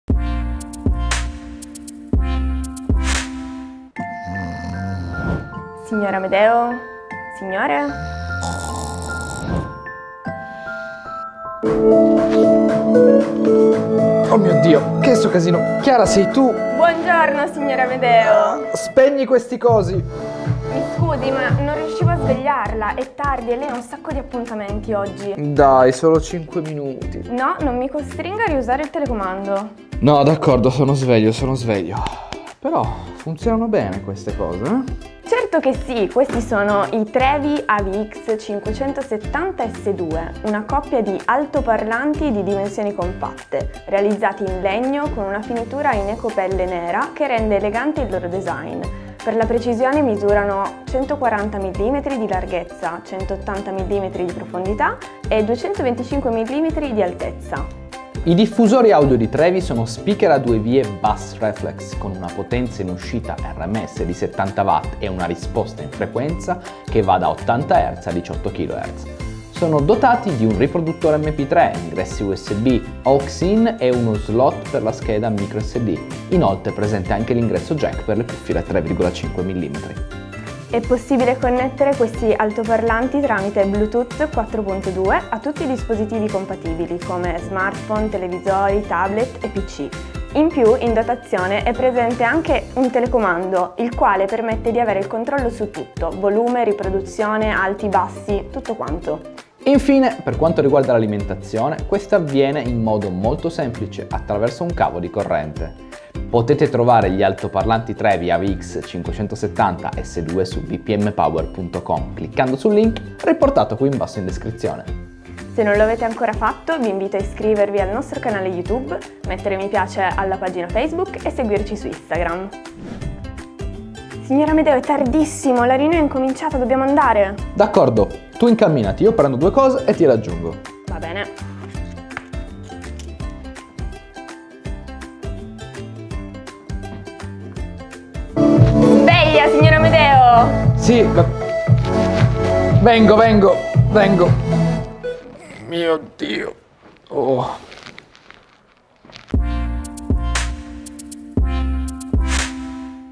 Trevi AVX 570 S2, altoparlanti economici ma buoni!
Vi parliamo un po' di Trevi AVX 570 S2, una coppia di altoparlanti di dimensioni compatte, realizzati in legno con una finitura in ecopelle nera che rende elegante il loro design. Per la precisione misurano 140 mm di larghezza, 180 mm di profondità e 225 mm di altezza.